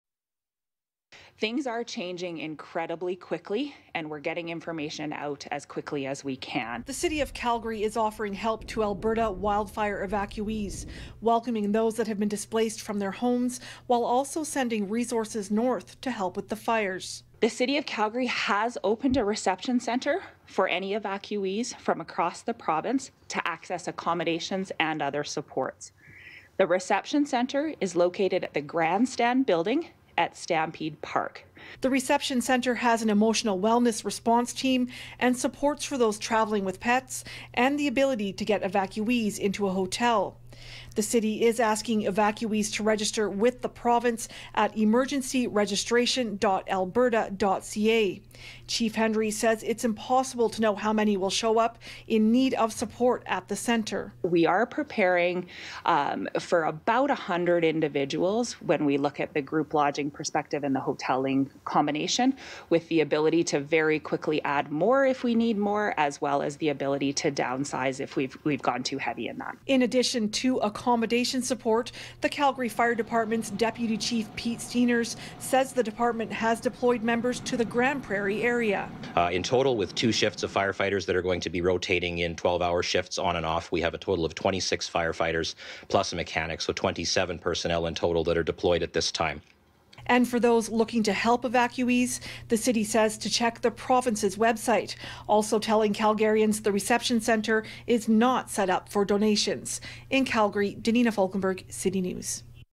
Calgary opens its doors to help Alberta wildfire evacuees
Alberta wildfire evacuees in need of accommodation will be offered help in Calgary.  CityNews’ Danina Falkenberg has more on the City of Calgary’s wildfire evacuee reception centre and the fire crews that have been sent to help battle the fires.